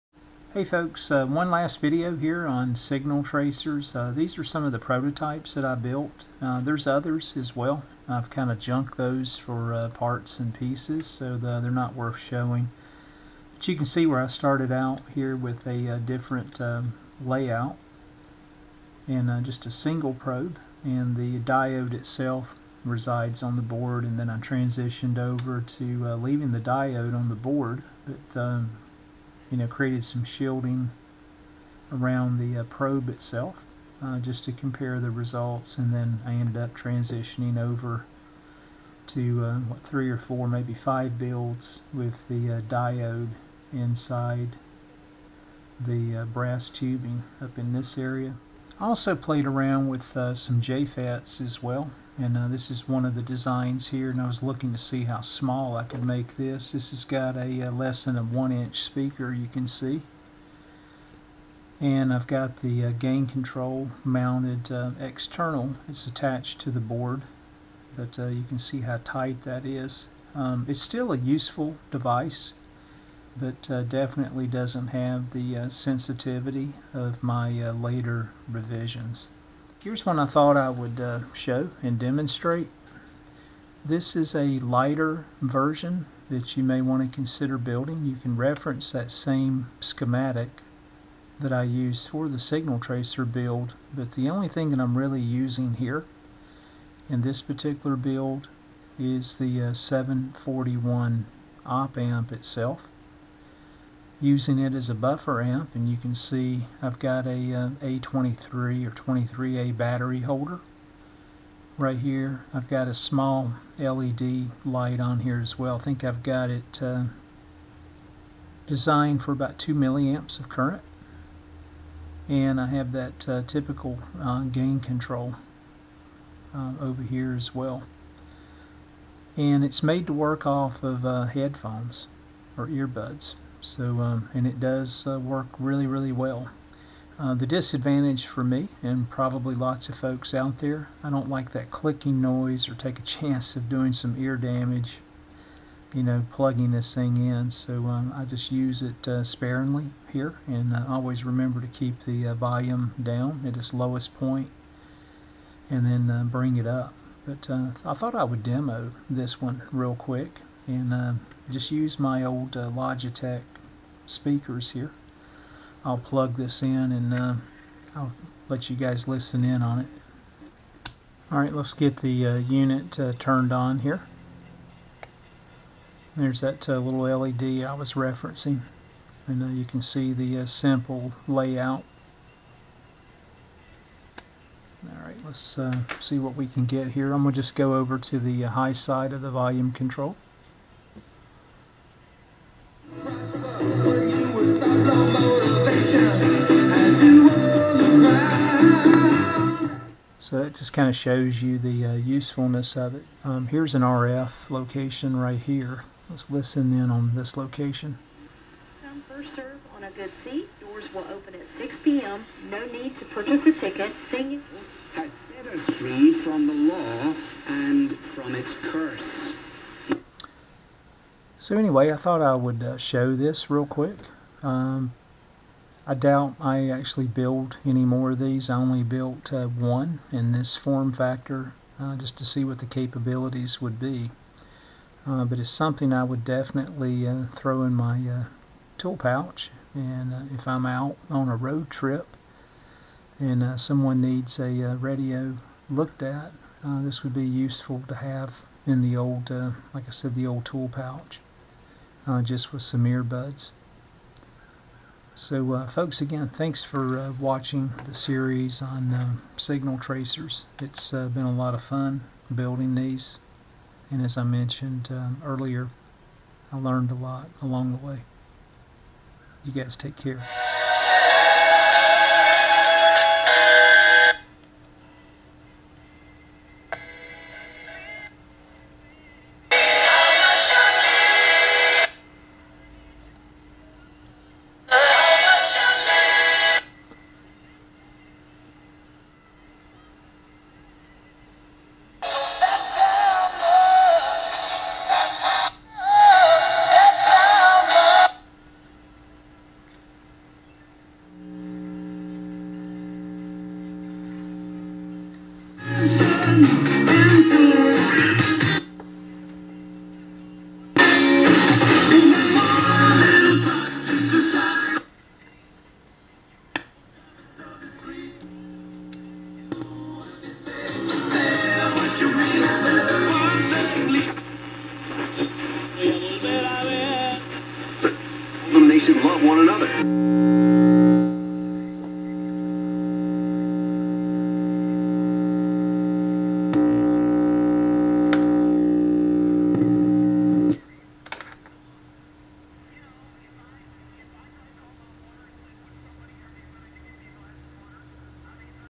Signal Tracers - A Simple Build & Look Back
A look back at some of my prototype builds after some time spent on the breadboard. In addition, a look at a simple, but effective signal tracer that can be used with earbuds or an external amplifier.